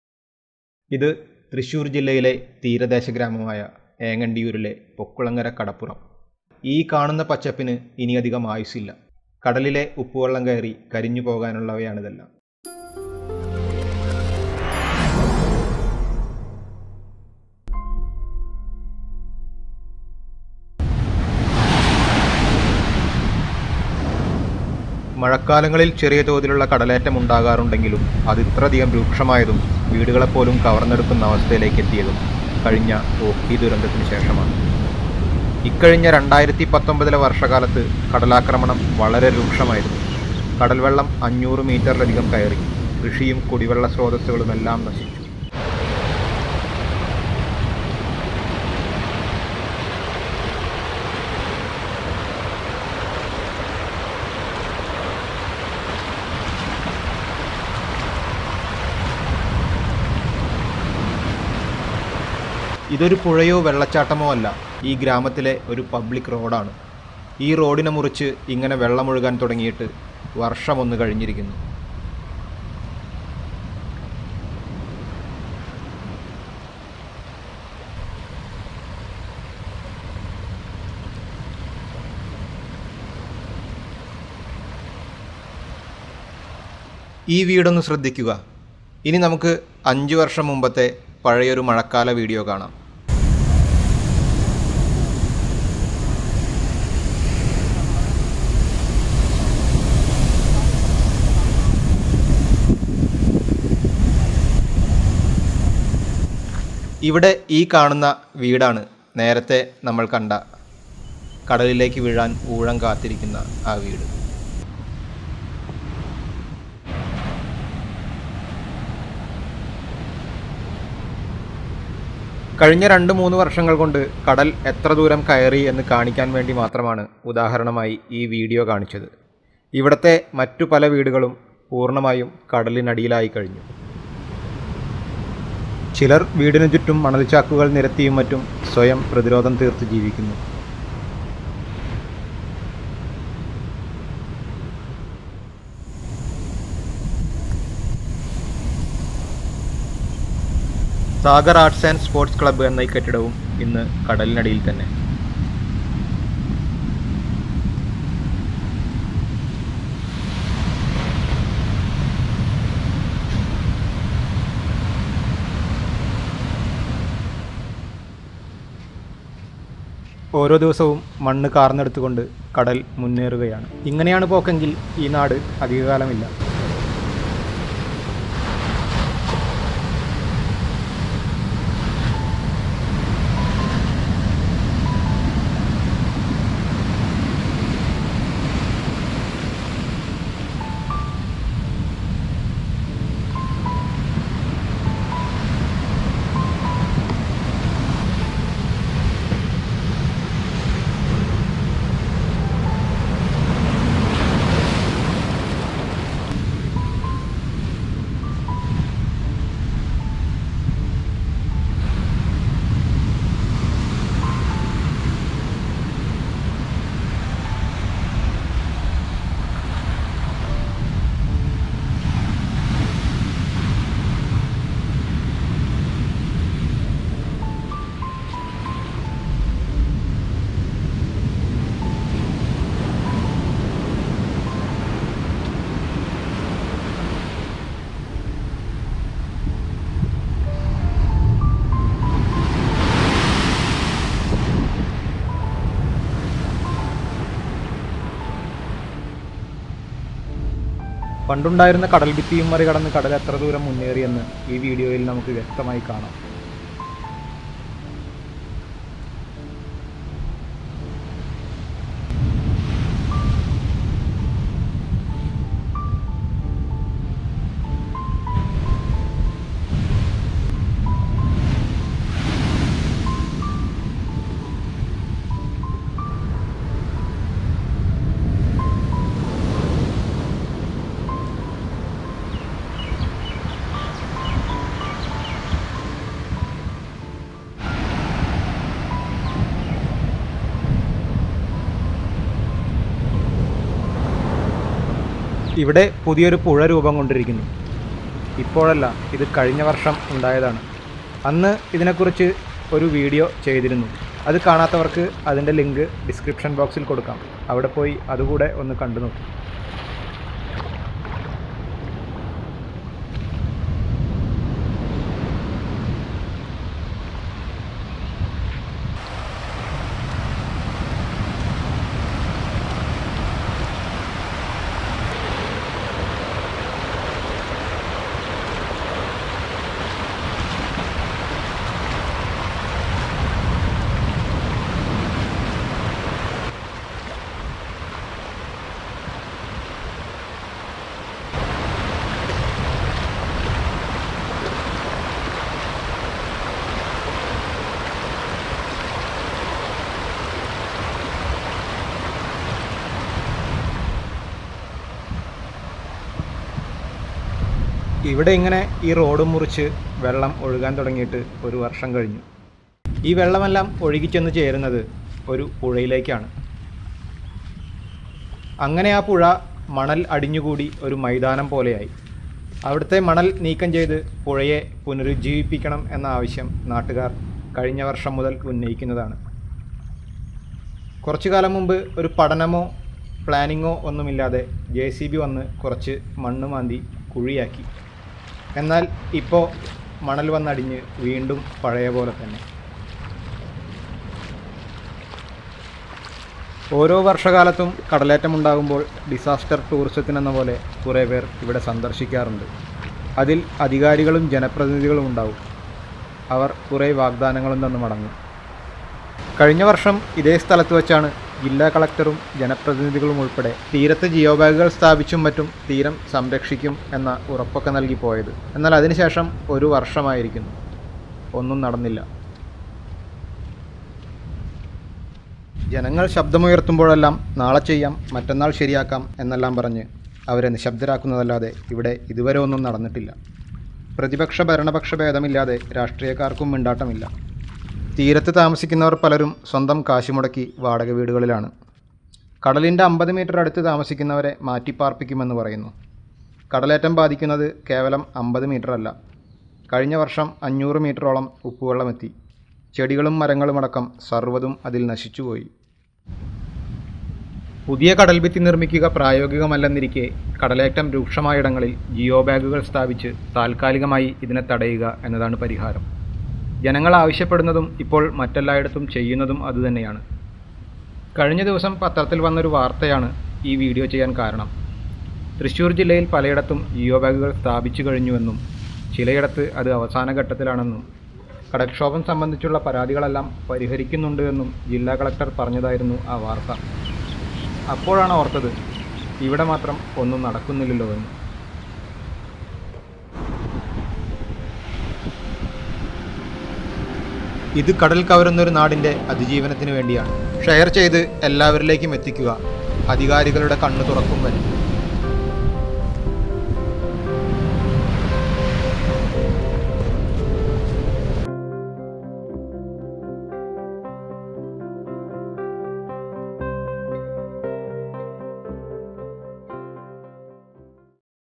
ഏങ്ങണ്ടിയൂർ - തൃശ്ശൂരിലെ ഈ ഗ്രാമം ഇല്ലാതാവുന്നു | The vanishing village
Engandiyur തൃശൂർ ജില്ലയിലെ ഈ ഗ്രാമത്തിന്റെ തീരപ്രദേശങ്ങൾ അതിവേഗം ഇല്ലാതായിക്കൊണ്ടിരിക്കുന്നു. കഴിഞ്ഞ രണ്ടു വര്ഷം കൊണ്ട് 100 മീറ്ററിലധികം പ്രദേശങ്ങൾ കടലിൽ മുങ്ങിക്കഴിഞ്ഞു.
അധികാരികൾ ഇനിയും തിരിഞ്ഞു നോക്കാത്ത ഈ നാട് ഭൂപടത്തിൽ നിന്ന് അപ്രത്യക്ഷമാവാൻ അധിക നാളുകളില്ല.